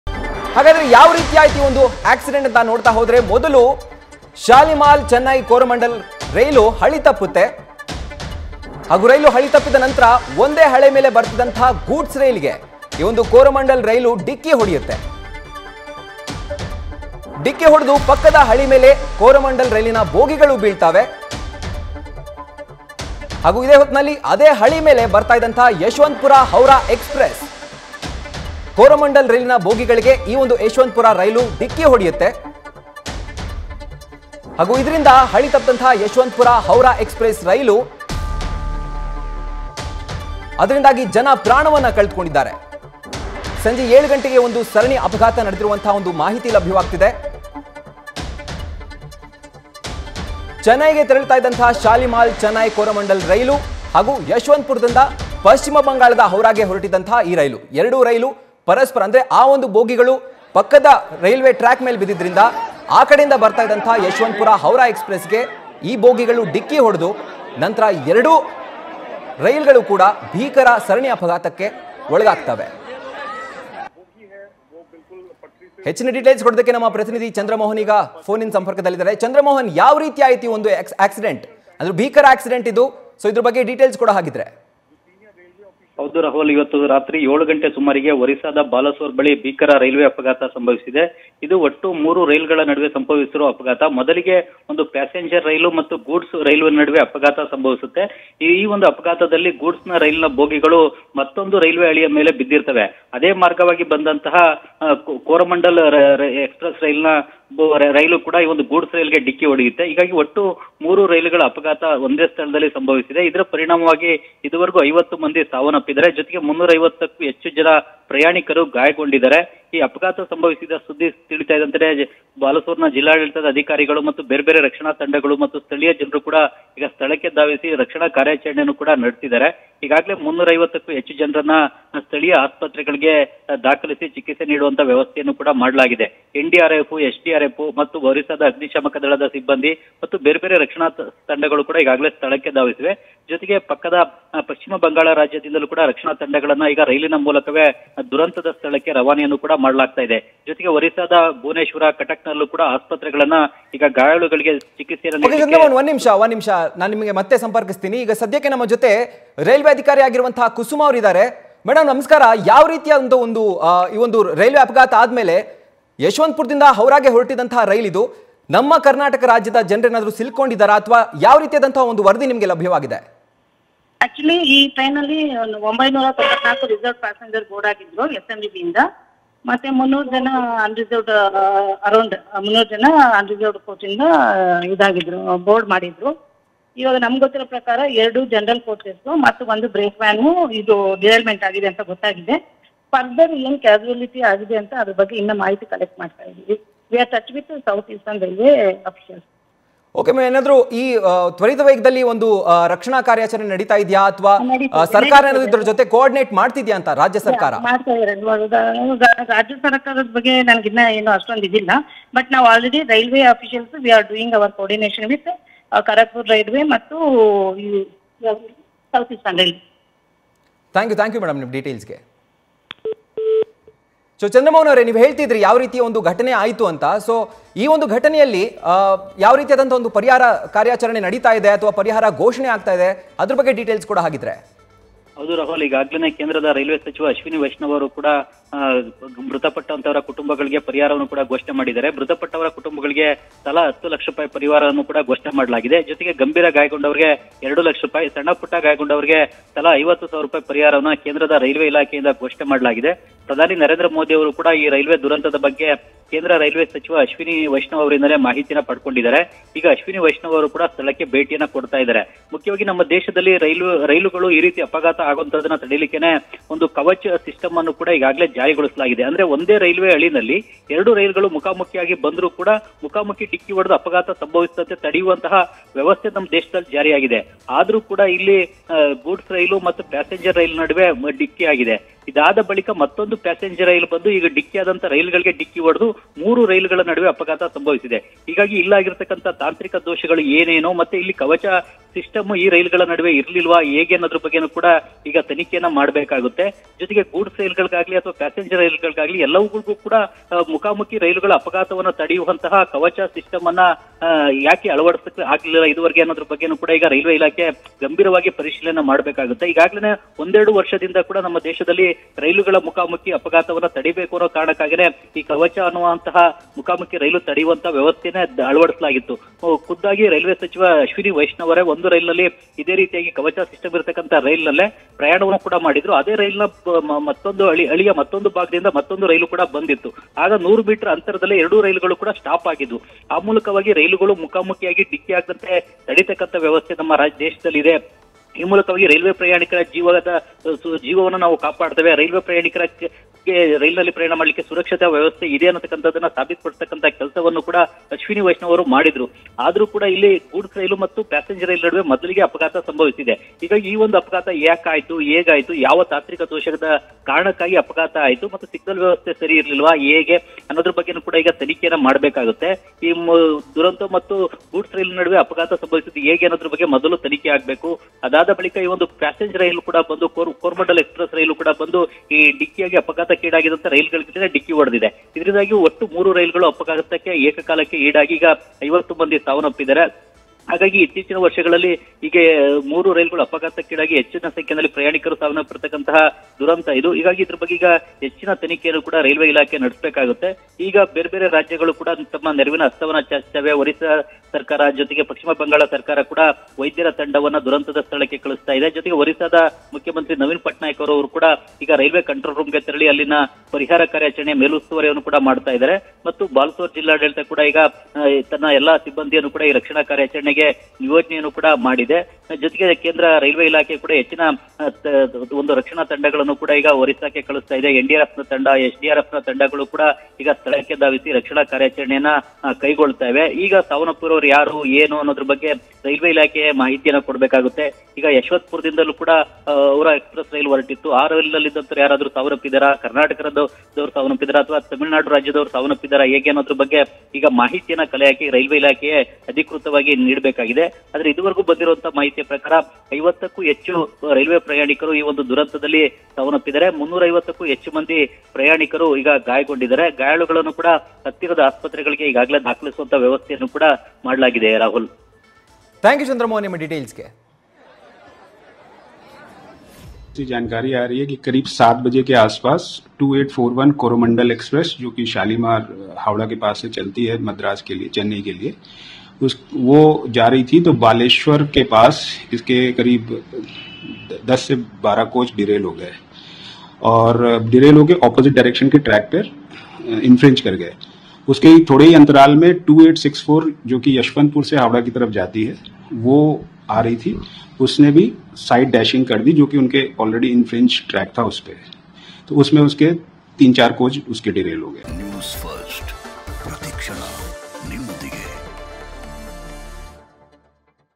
Coromandel Express : 3 ರೈಲುಗಳ ಮಧ್ಯೆ ಸರಣಿ ಅಪಘಾತ ನಡೆದಿದ್ದು ಹೇಗೆ? | Odisha Train Incident | Newsfirst
Coromandel Express : 3 ರೈಲುಗಳ ಮಧ್ಯೆ ಸರಣಿ ಅಪಘಾತ ನಡೆದಿದ್ದು ಹೇಗೆ? | Odisha Train Incident | @newsfirstkannada